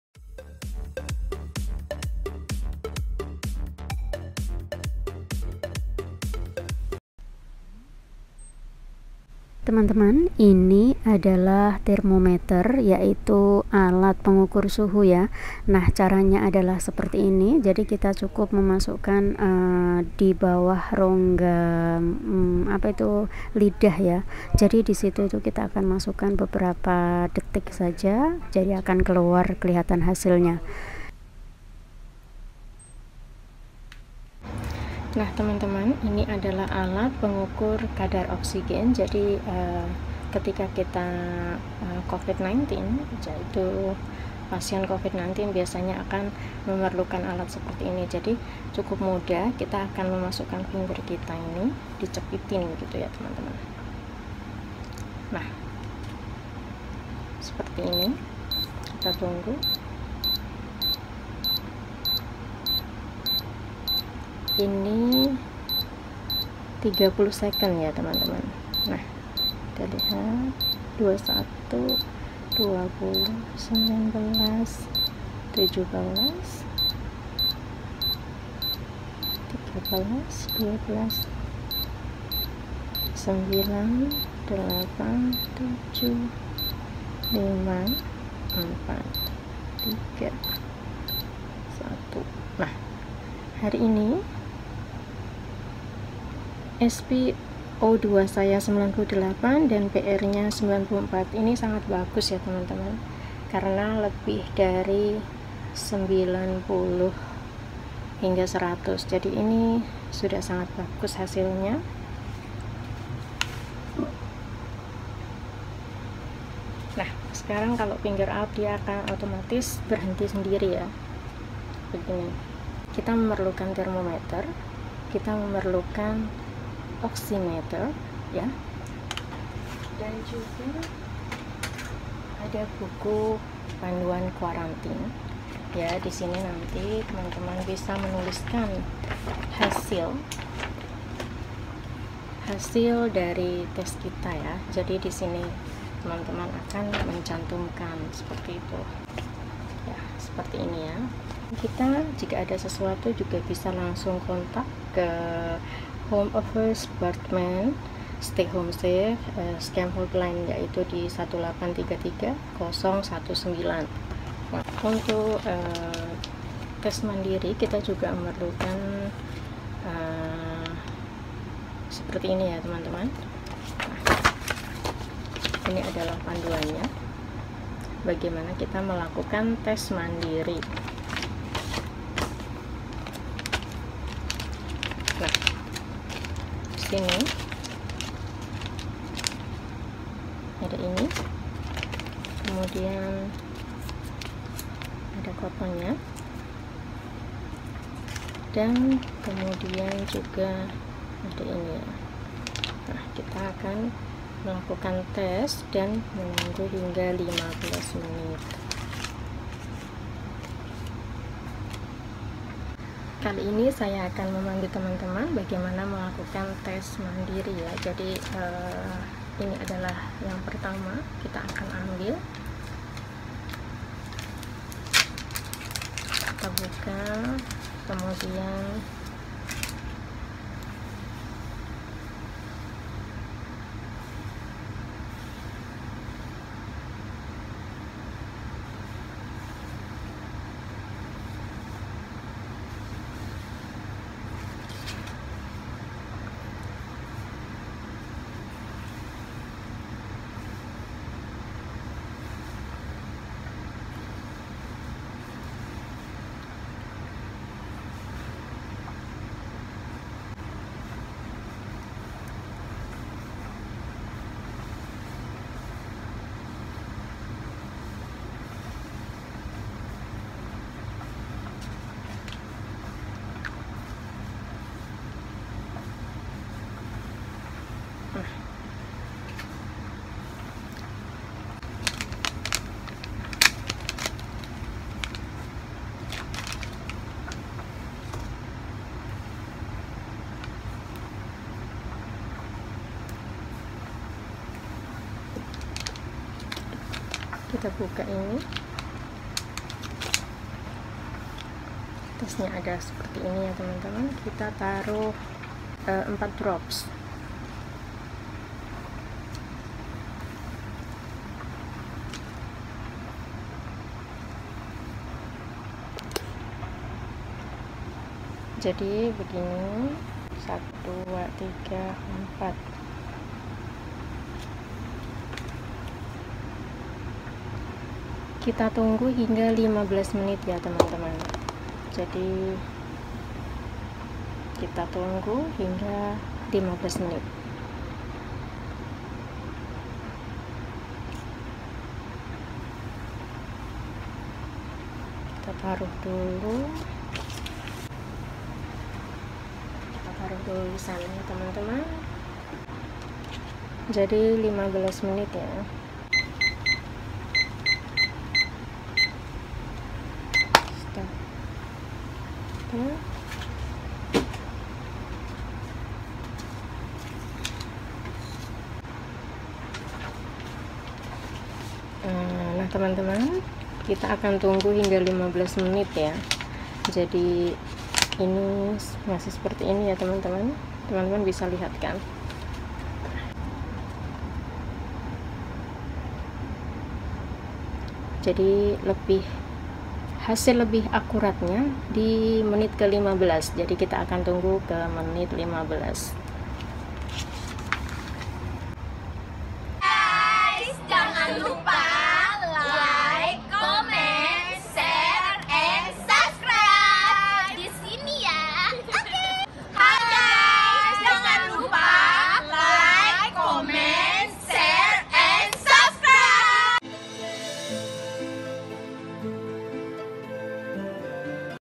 🏡How to use,”OXIMETER ,TERMOMETER,& RAPID TEST”|| when having Self quarantine at home
Teman-teman….
Ini adalah alat yang saya gunakan saat KARANTINA Mandiri Di rumah majikan ya..😊🙏🏾
🌻OXIMETER
🌻TERMOMETER
🌻Rapids  Test

Hong Kong has taken the unprecedented move of allowing close contacts of Covid-19 patients to be isolated at home in a bid to ease pressure on quarantine facilities.

The authorities will assess if the person’s home is suitable for quarantine, including whether he or she lives in a subdivided flat or must share a kitchen and toilet with others.

The government on Wednesday (25 Mei 2022) said it had ordered compulsory testing for Covid-19 at 42 locations across the SAR.

Anyone who has visited any of these premises and stayed there for the length of time and on the dates specified must get a nucleic acid test for Covid-19.

The Centre for Health Protection reported 251 new Covid infections on Wednesday.